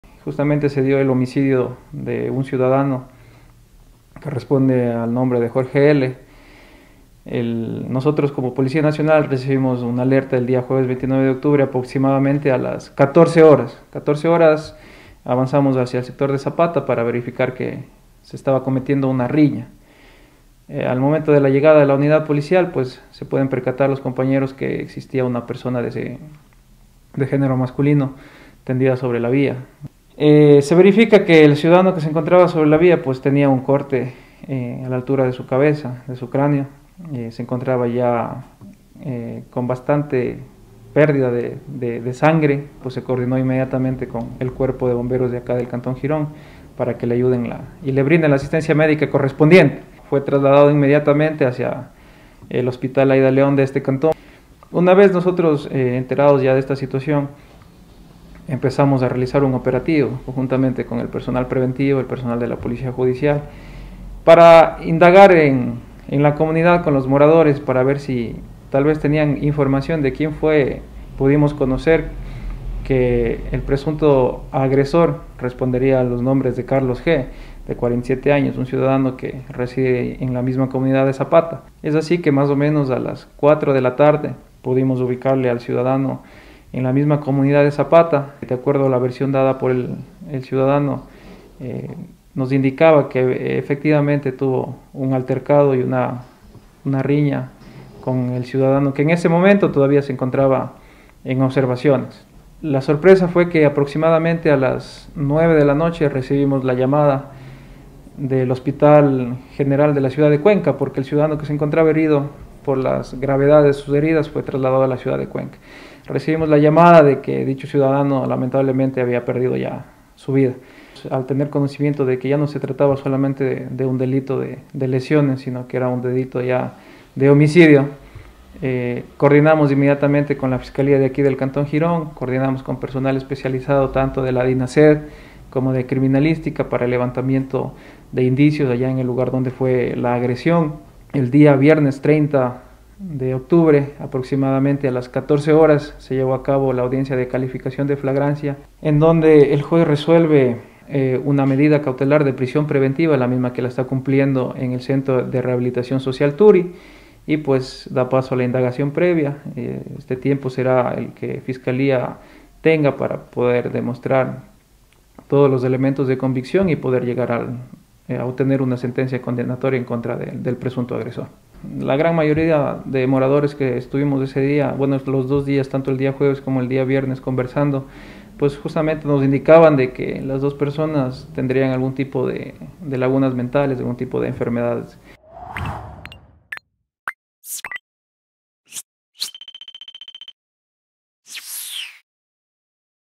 Probablemente a causa de una riña un ciudadano en Zapata perdió la vida
El jueves 29 de octubre, en la comunidad de Zapata se registró un acto violento que involucró a dos personas de sexo masculino, a consecuencia de lo cual, uno de ellos posteriormente, en una casa de salud perdió la vida. El presunto culpable está en prisión preventiva hasta que se esclarezca el caso.